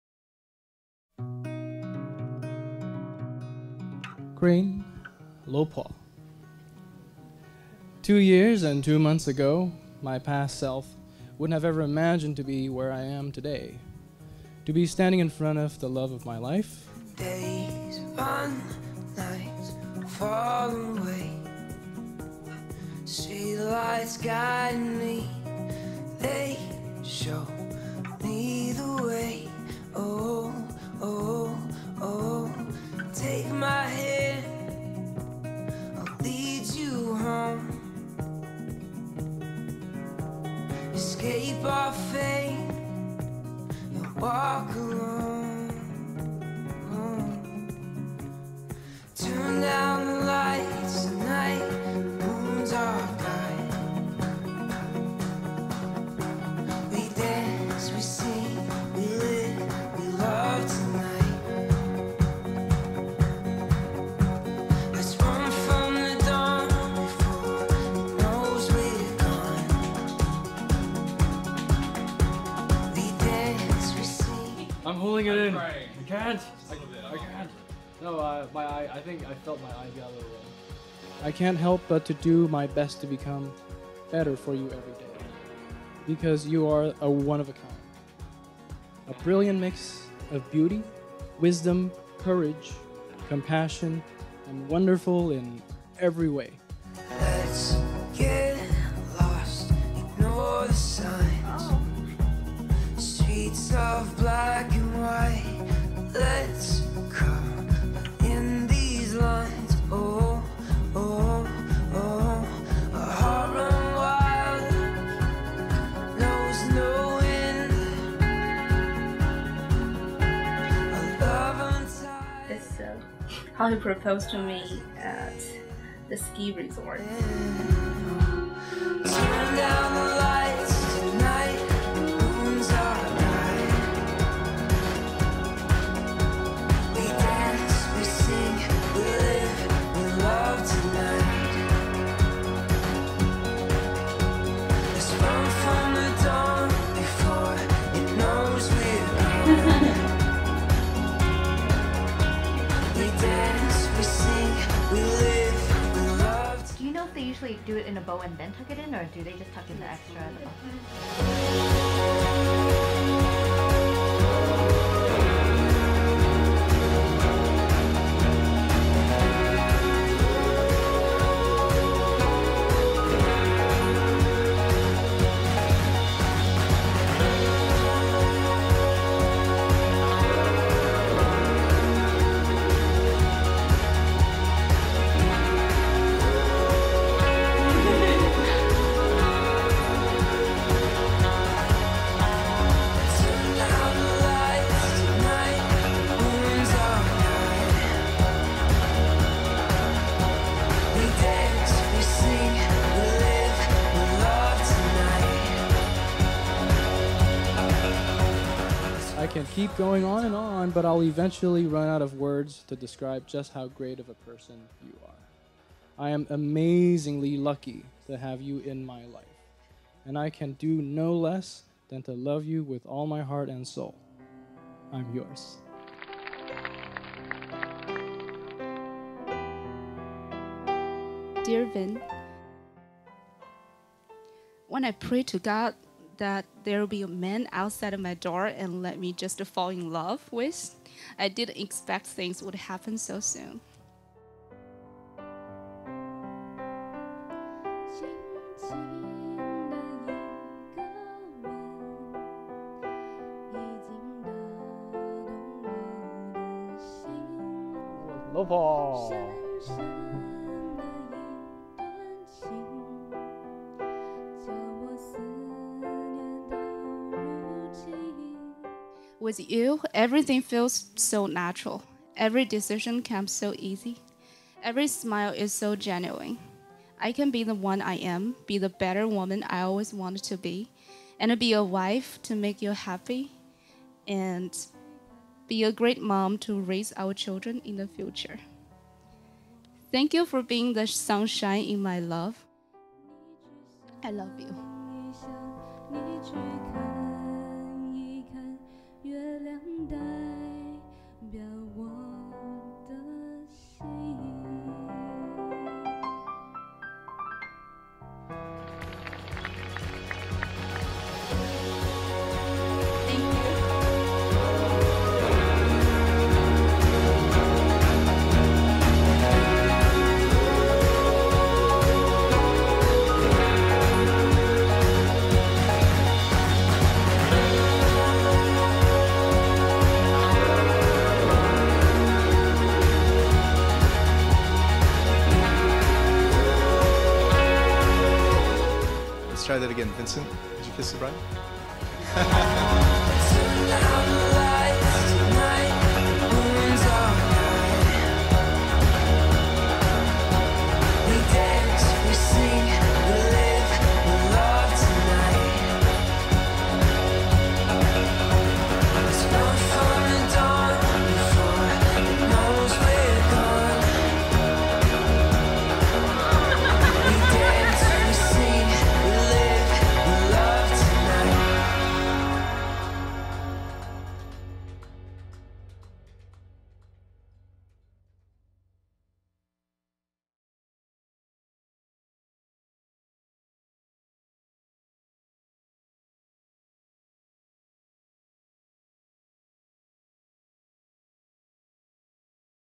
Crane and Vincent HD
One of the most things that we always want in a wedding is content where we can make a more evolving love story.
At Crane and Vincent's wedding, we had so much of that that it was easy to find and build the film story.
Vincent put so much time and effort in his wedding gift to Crane that we had to use it for their film cover.
I wish all the best to Crane and Vincent and this new step of their lives.